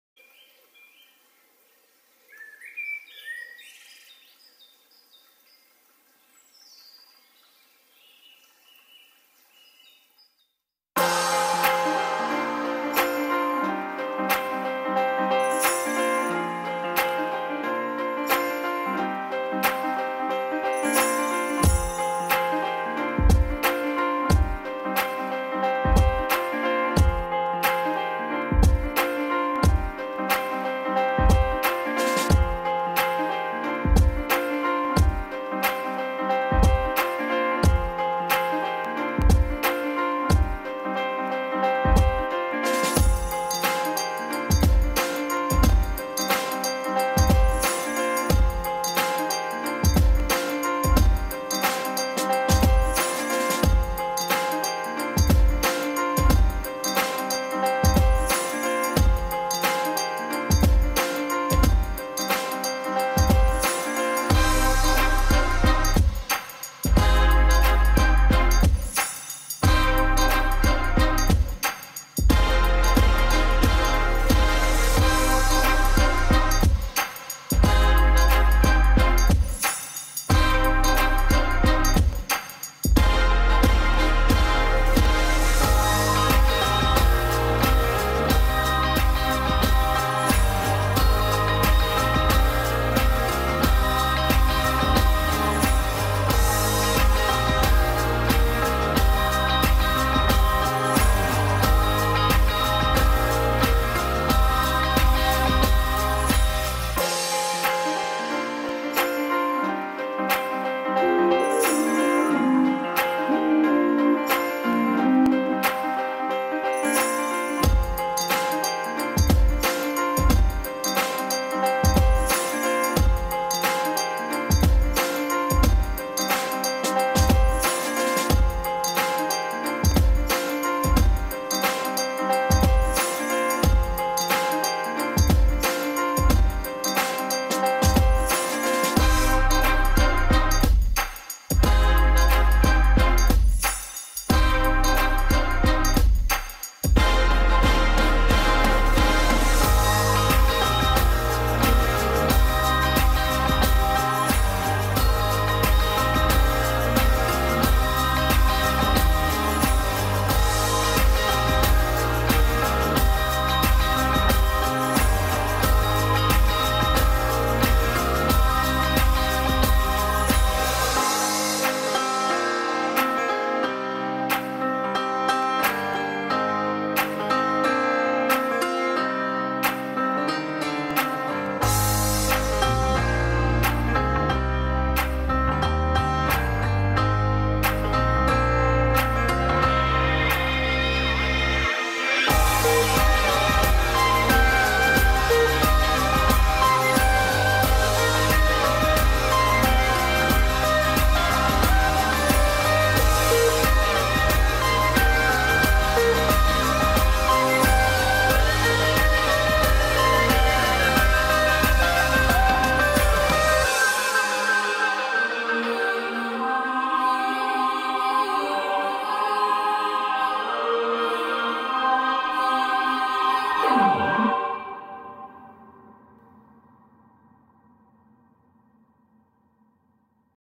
Camping & Fishing at Raystown Lake
These are some the clips taken from our recent camping / fishing trip to Raystown Lake in Pennsylvania, this is also my first attempt at a more cinematic video so please feel free to leave me any feedback in the comments.